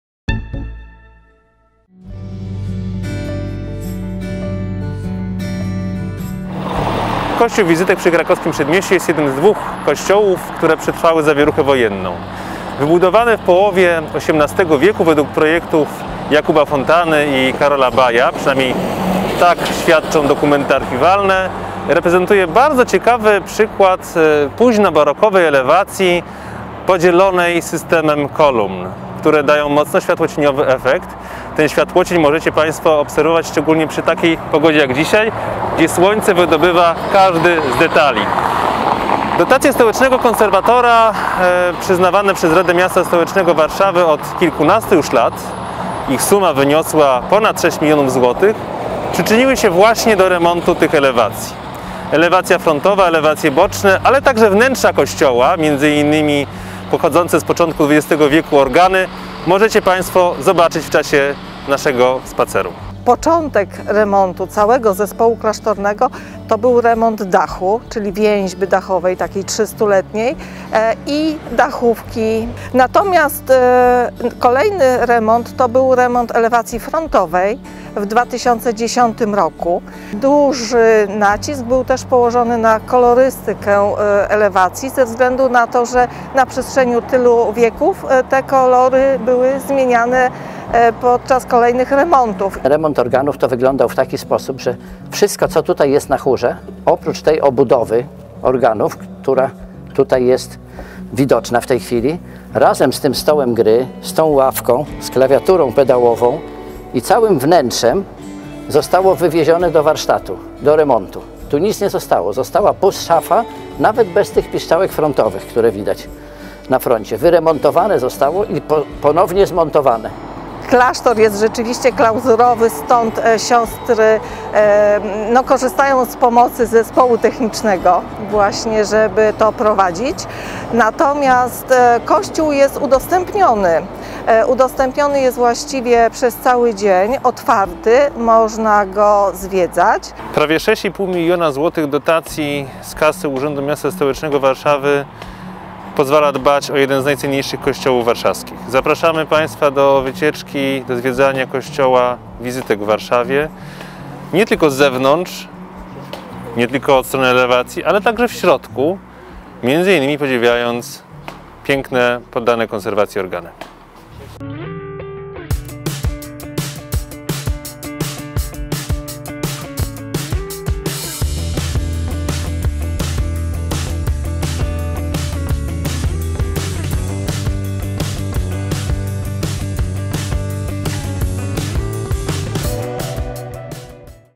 Spacery "Warszawskie konserwacje" - kościół Wizytek
Od kilkunastu lat warszawskie zabytki pięknieją dzięki dotacjom miasta. Efekty remontów możecie podziwiać podczas spacerów „Warszawskie Konserwacje”. W tym roku, z powodu epidemii, formuła wycieczek uległa zmianie. Zapraszamy do obejrzenia wyremontowanych zabytków online. Przed nami video o remoncie kościoła Wizytek przy Krakowskim Przedmieściu.